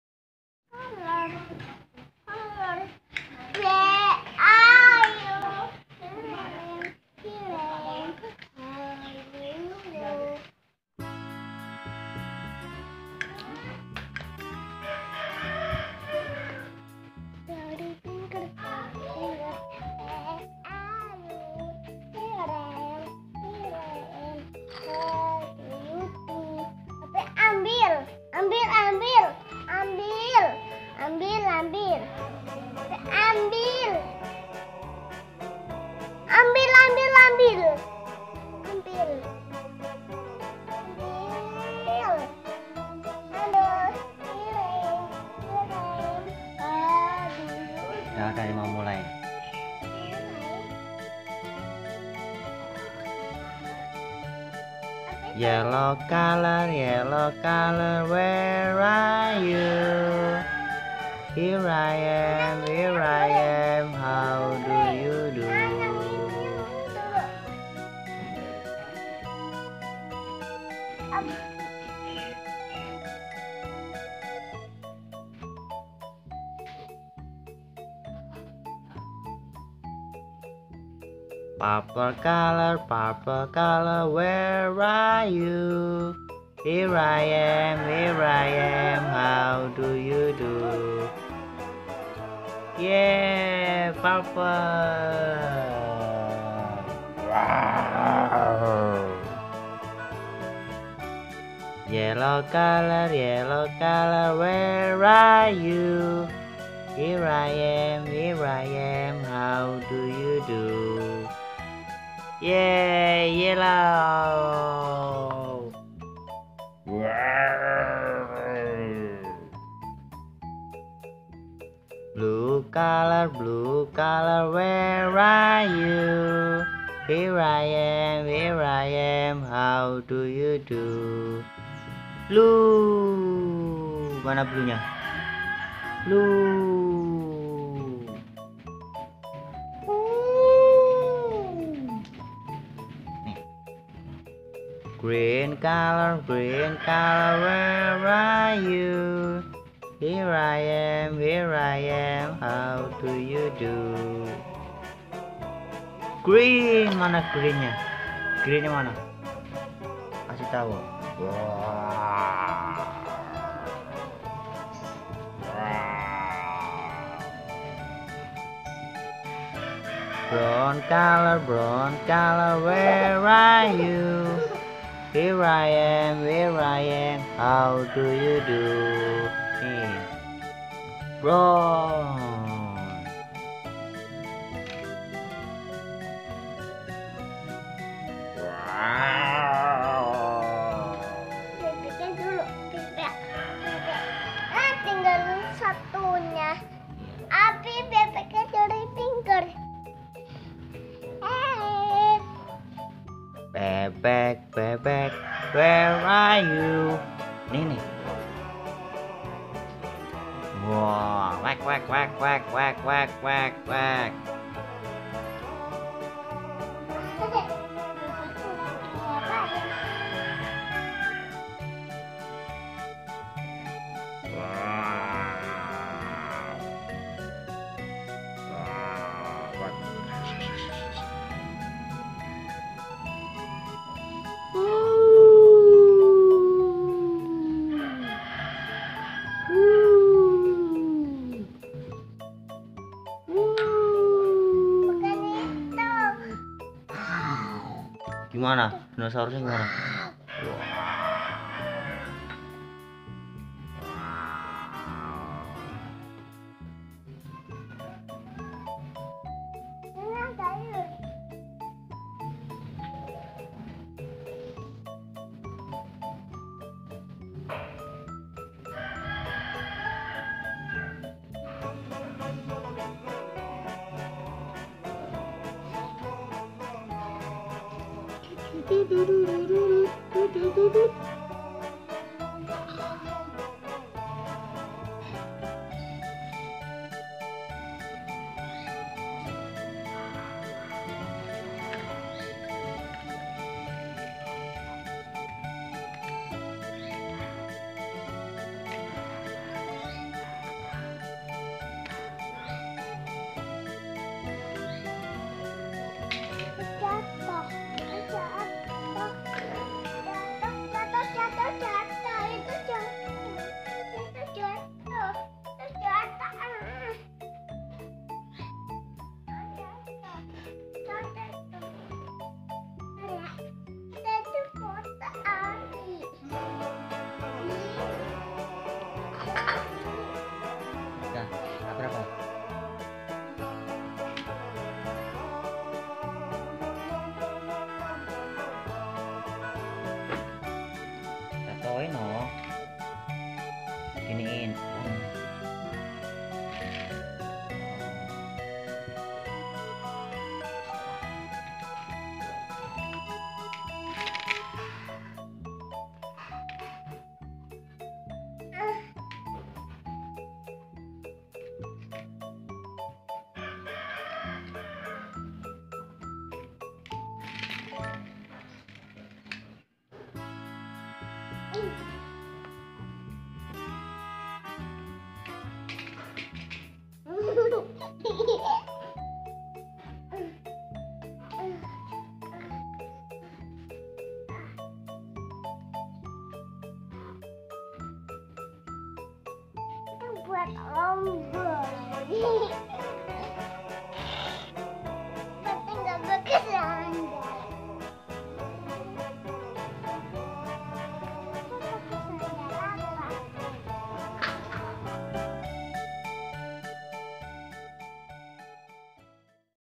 Learning colours with Dinosaurs | Dinosaurus
Welcome to learn colors With dinosaurs for kids and babies.
Belajar Mengenal Warna untuk anak anak dengan Dinosaurus.